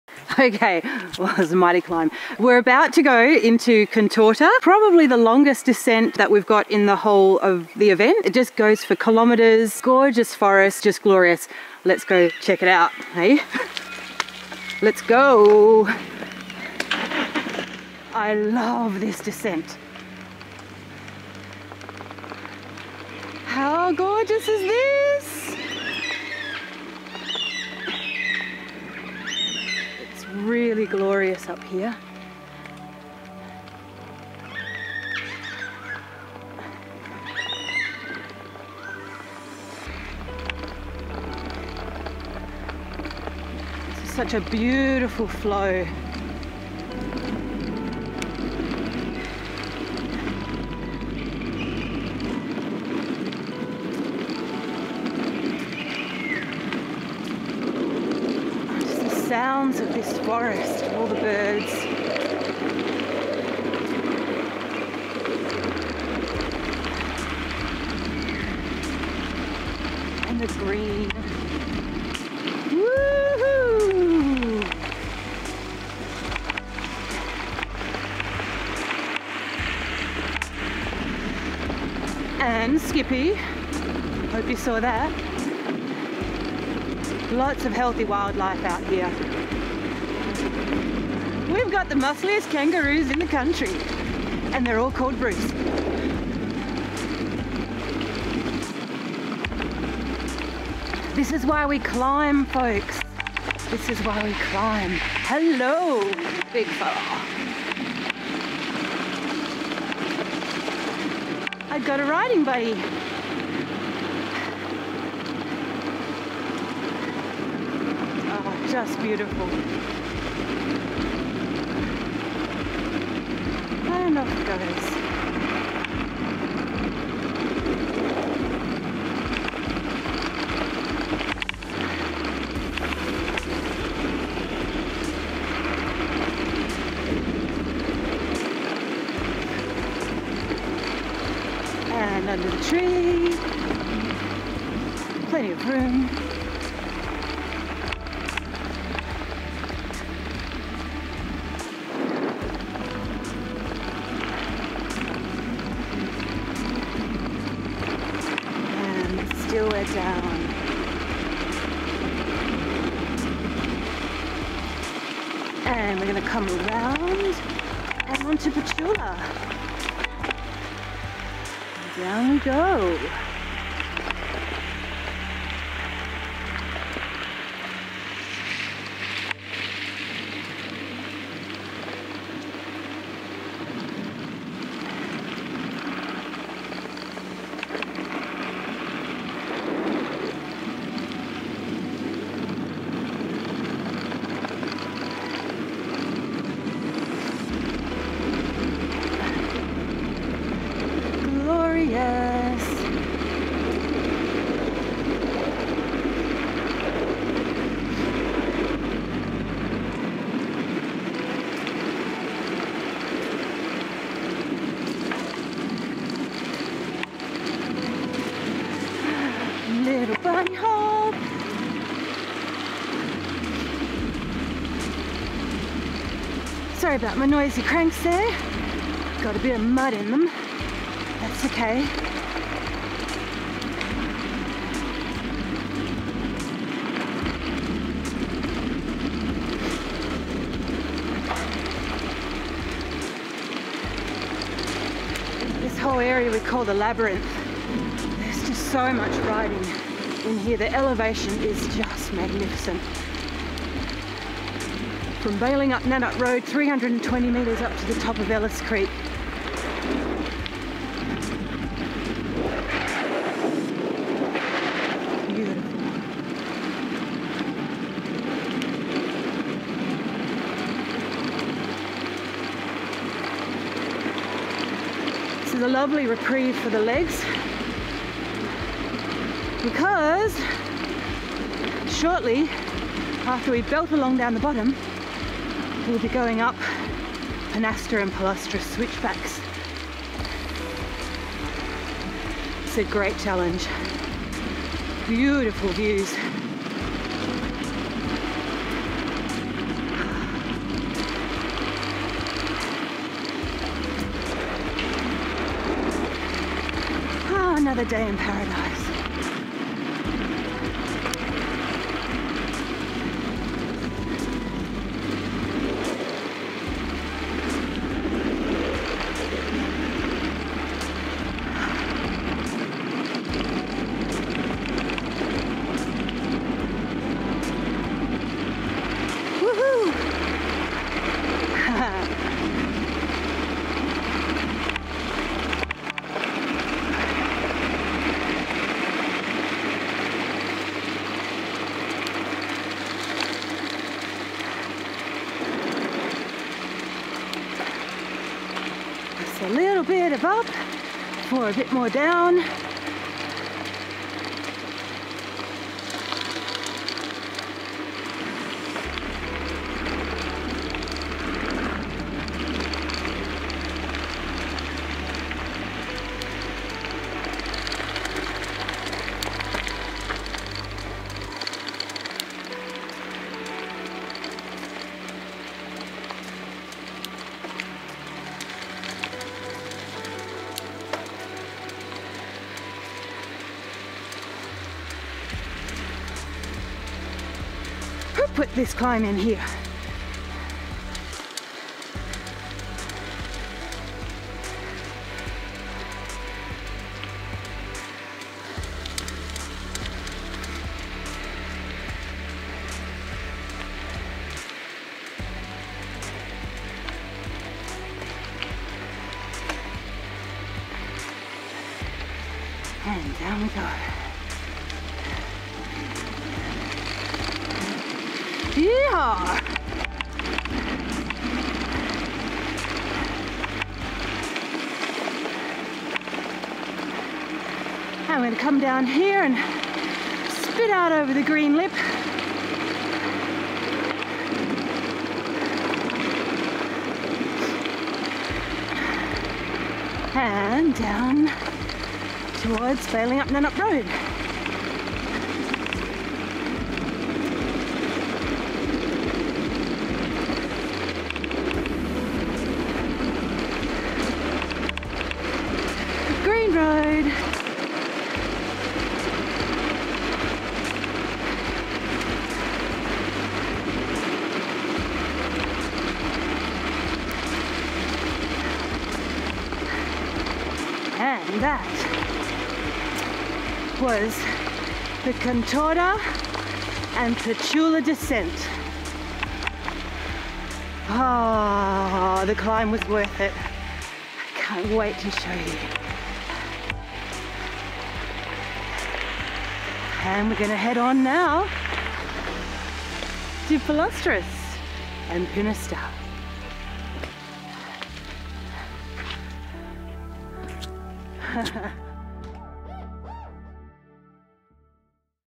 DIRT 2019: The Labyrinth
The hypnotic, flowing descent of Contorta Road. For every one of the twenty categorised climbs in DIRT there is a reward on the other side of the summit. Enjoy.